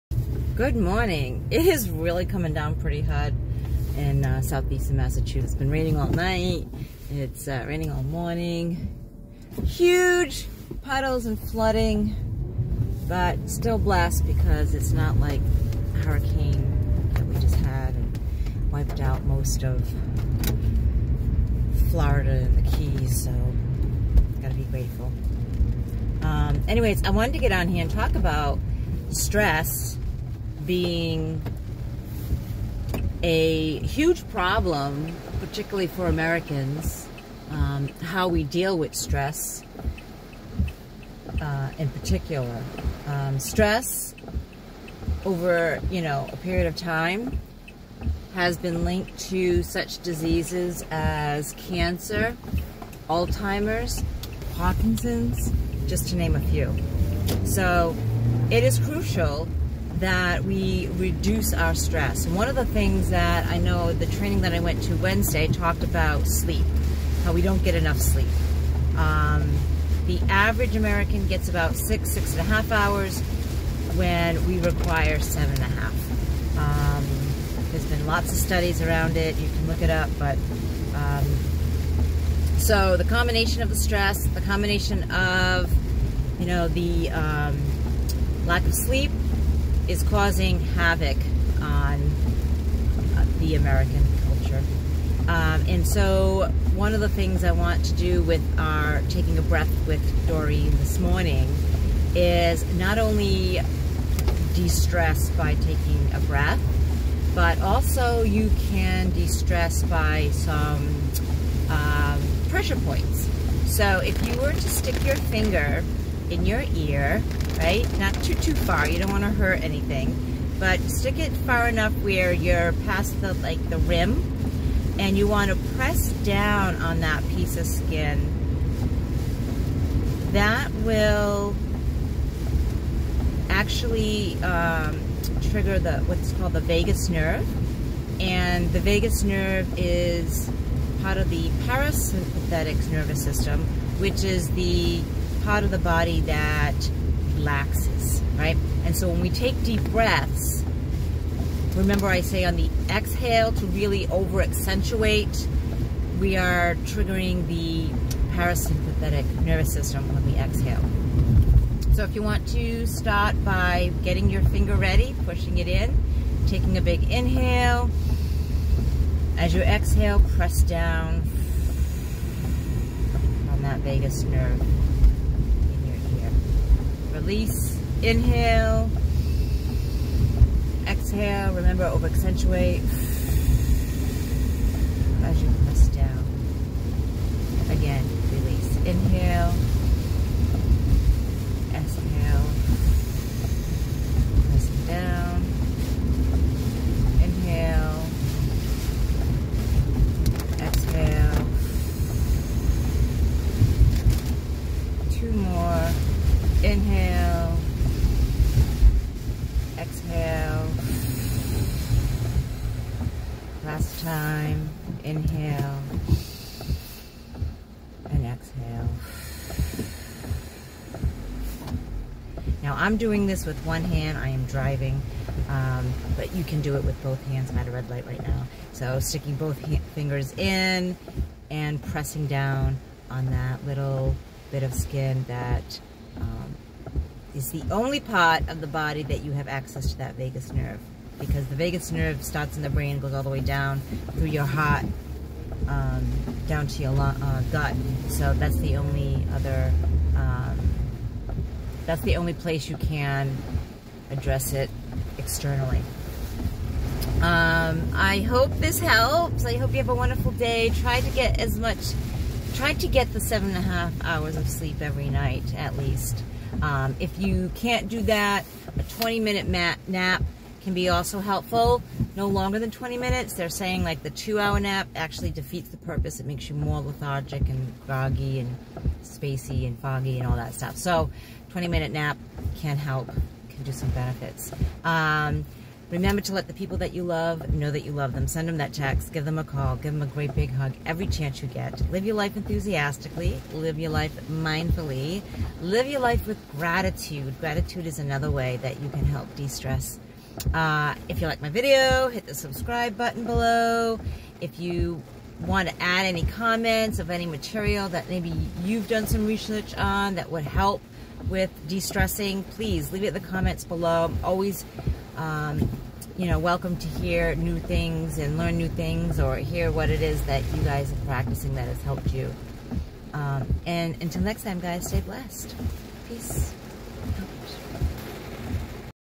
Lack of sleep is causing havoc
Stress is linked to many disease, many preventable diseases
Sleep or lack there of increases stress which increases these diseases.
Wouldn't it be wonderful if we could decrease the risk and experience less stress?